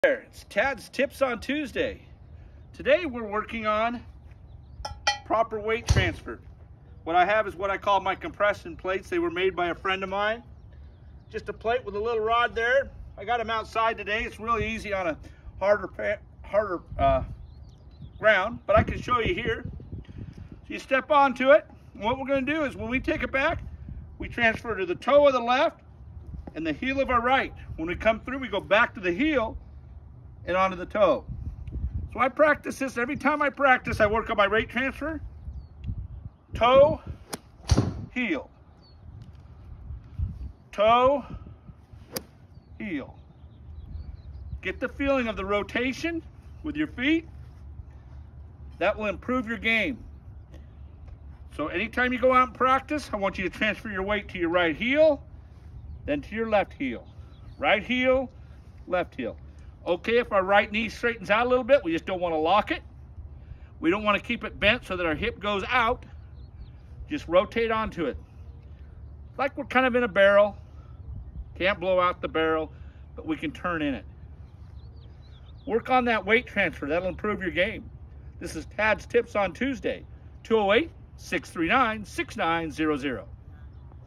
How to Transfer Proper Weight to Your Golf Swing
Use this drill to work on ensuring proper weight transfer ⛳ 🏌️‍♂️